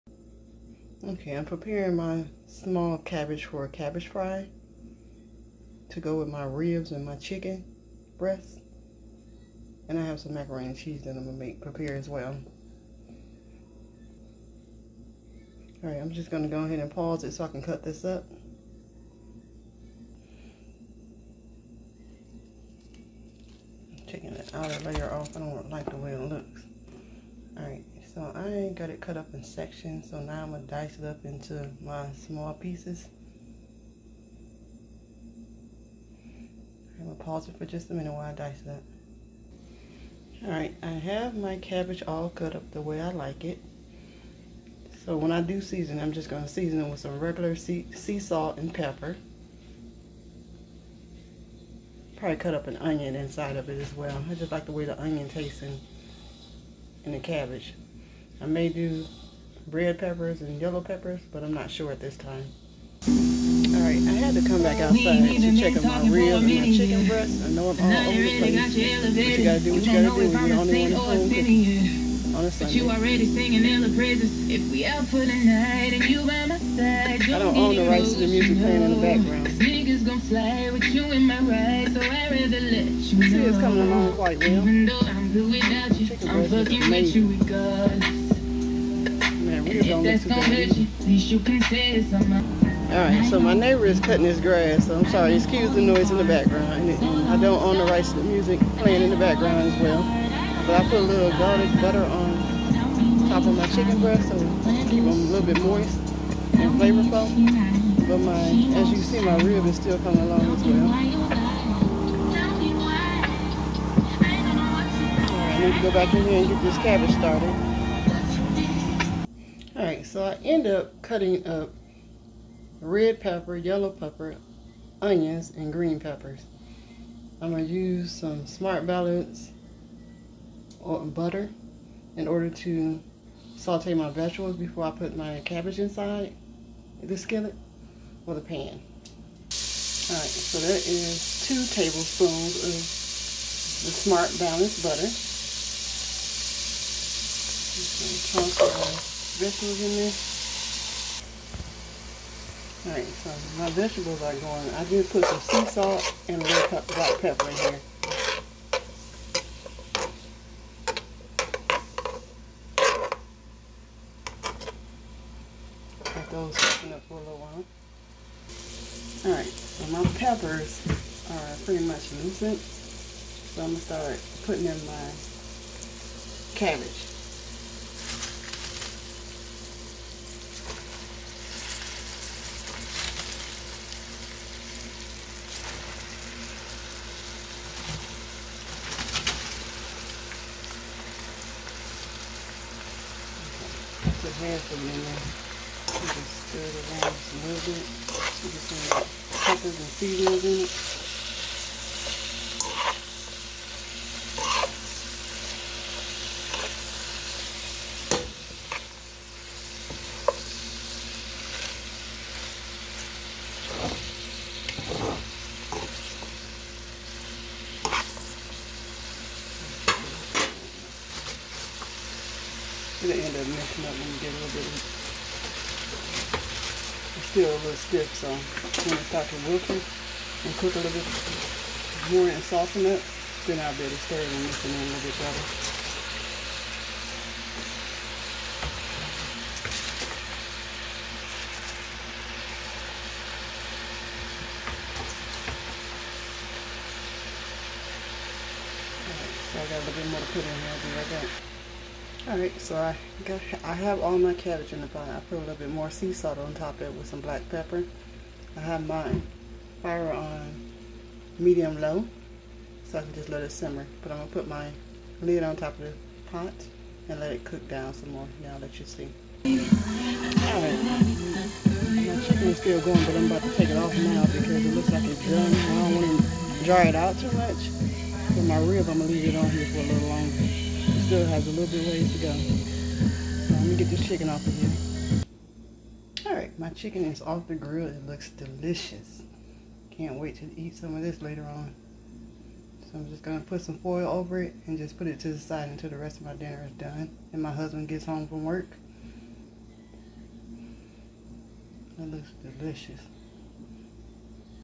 Prepping Fried Cabbage, Rib and Chicken Breast on the grill
Part 4, Sunday meal consist of Rib, Chicken Breast and fried Cabbage.